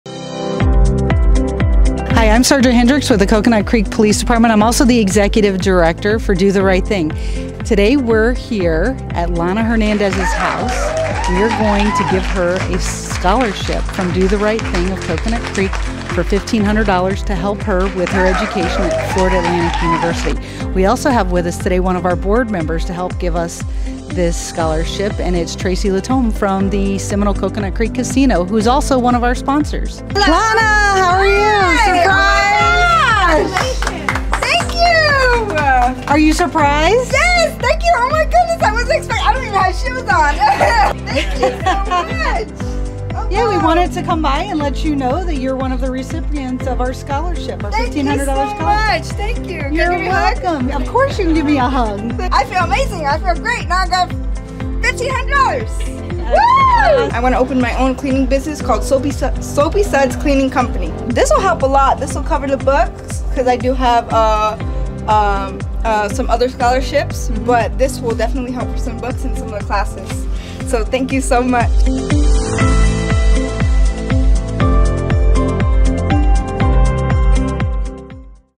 DTRT Scholarship Winner Lahna Hernandez is heading to FAU
Do The Right Thing of Coconut Creek caught Lahna Hernandez in her bare feet! Several members of Do The Right Thing recently surprised her with a $1,500 scholarship. She is one of the organization's scholarship award winners for this past school year. Recognized as a Do The Right Thing of Coconut Creek honoree back in October 2021 for finding a wallet and turning it in, the recent Atlantic Tech High School graduate will start Florida Atlantic University in the fall. Meet Lahna in this fun video: